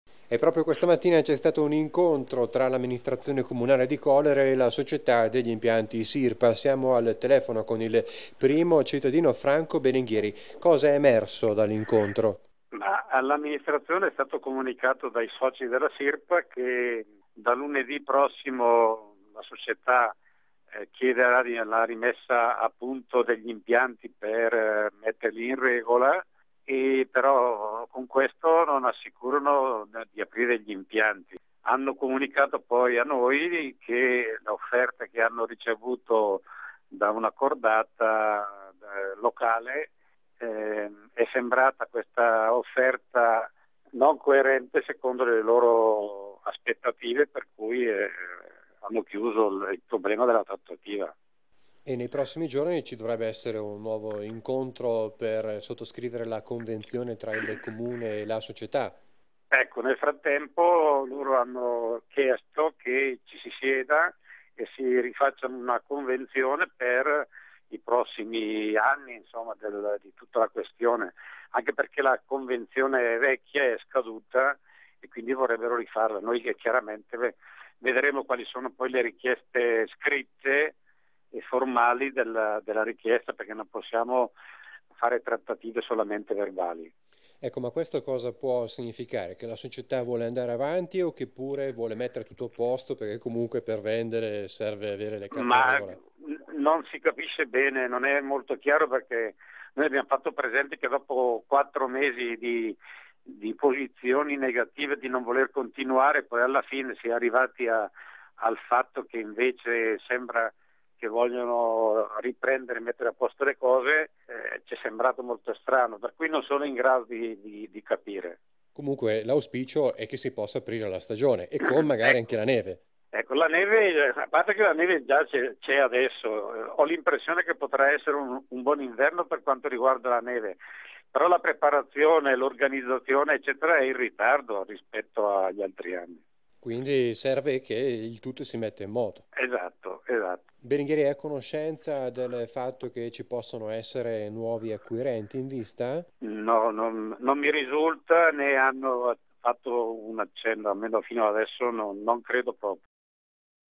Colere impianti sci salta la trattativa 5 novembre giornata decisiva Antenna 2 TV 03112012
Sembra proprio che lunedì (5 novembre 2012) sia una giornata decisiva per più realtà economiche del nostro territorio. Oltre all'incontro con le banche per quanto riguarda l'Honegger, nel primo giorno della prossima settimana si deciderà la riapertura o chiusura degli impianti di Colere. Oggi (sabato 3 novembre 2012) la Sirpa ha incontrato l'amministrazione comunale. Sentiamo cosa ci ha riferito il sindaco Franco Belingheri.